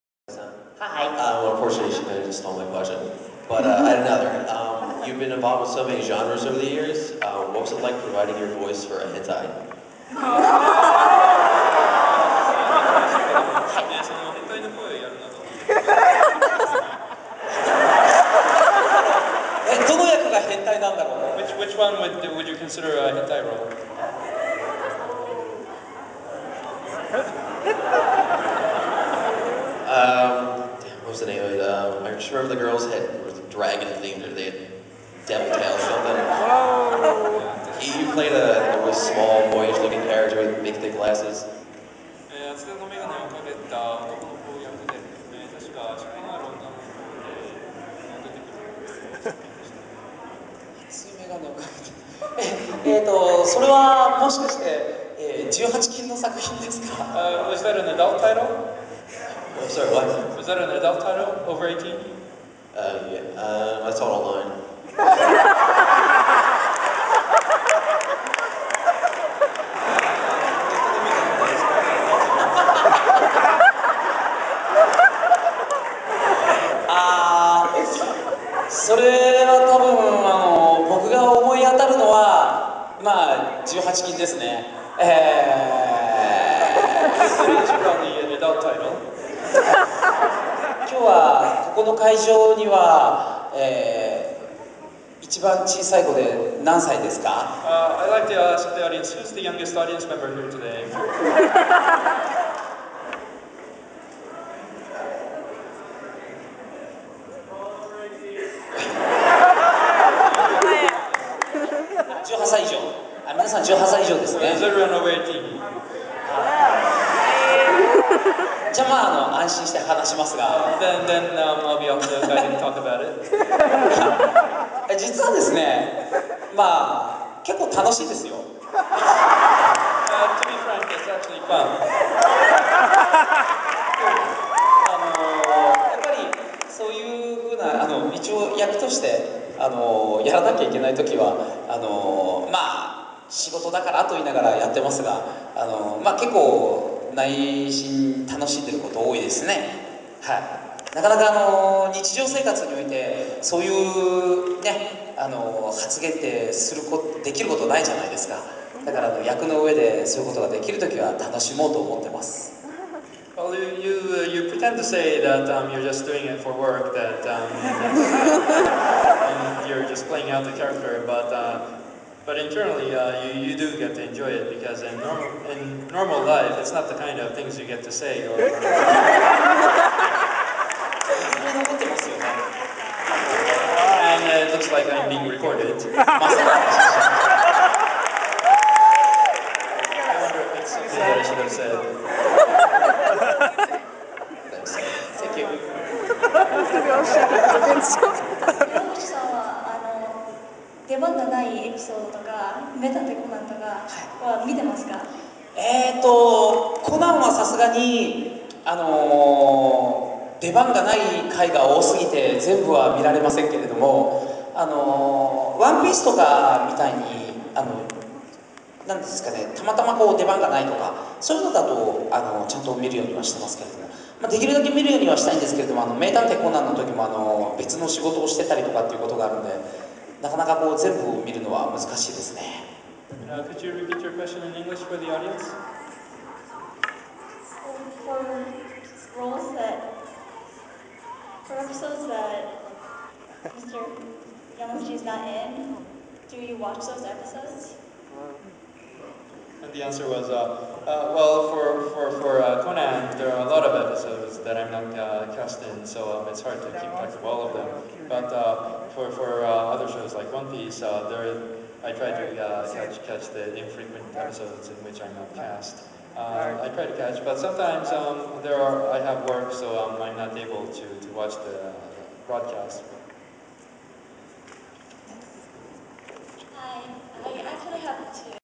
Otakon 2008-- Kappei Yamaguchi--"What's it like voicing hentai?"
Kappei-san is asked by a curious fan what it is like to lend his voice to the hentai genre at an Otakon 2008 discussion panel.